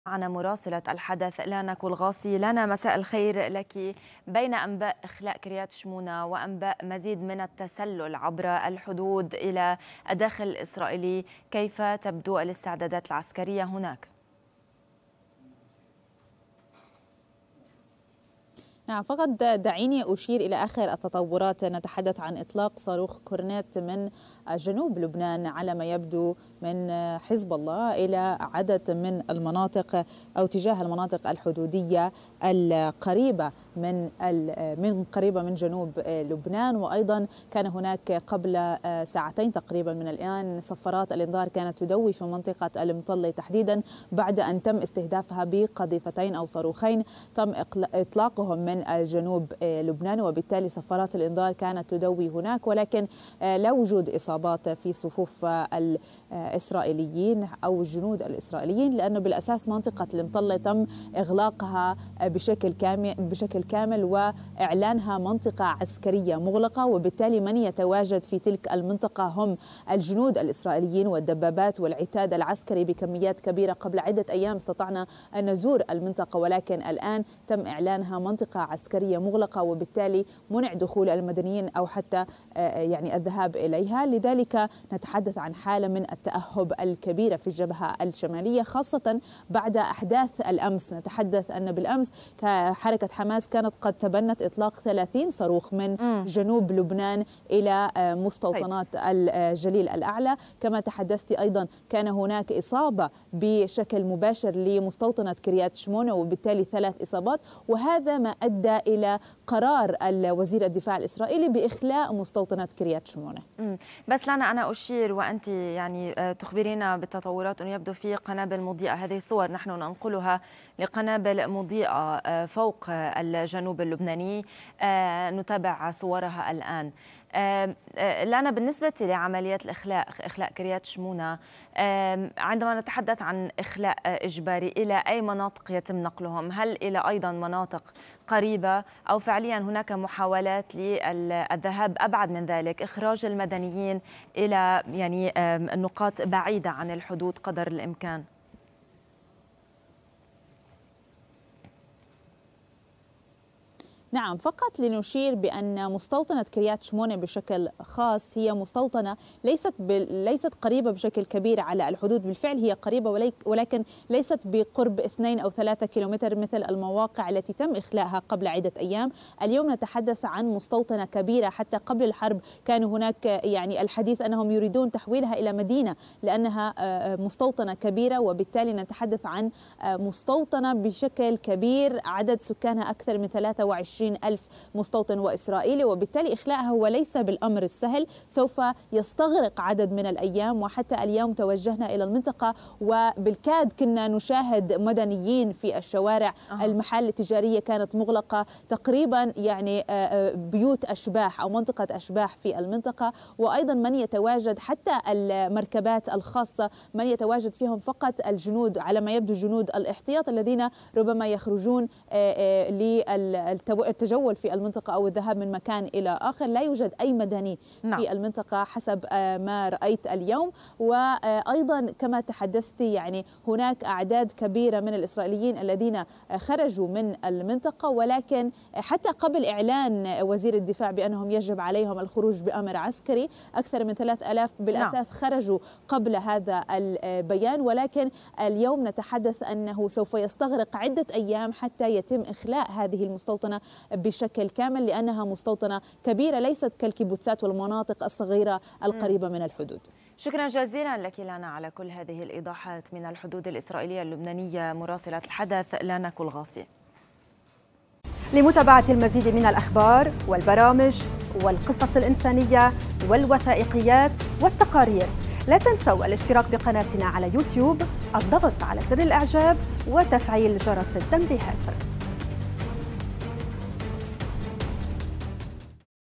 سكانها 23 ألفا.. إسرائيل تعلن خطة إجلاء مستوطني "كريات شمونة"
مراسلة الحدث في لبنان لانا كلغاصي: إجلاء إسرائيل لمستوطني "كريات شمونة" المحاذية لـ لبنان قد يستغرق أياما إذ يسكنها أكثر من 23 ألفا
لمعرفة أحدث المستجدات.. عبر البث المباشر من هنا ⤵
وبإمكانكم متابعة الحدث على منصات التواصل